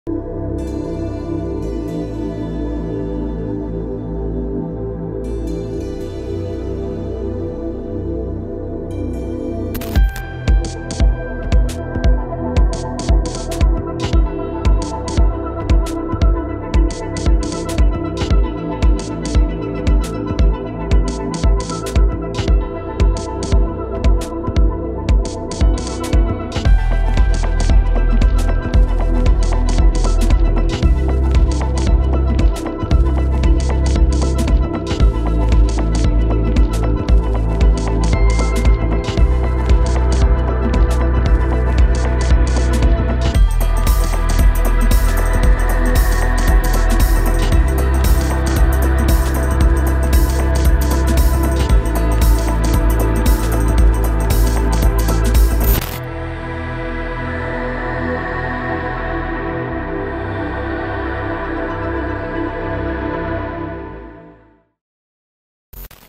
The SOUTH WEST of SWEDEN is BEAUTIFUL! Välkommen till BORSTAHUSEN (Skåne)
The South West of Sweden has some beautiful scenery and coastline, so here's a little sneak peak of the area in which I've chosen to live!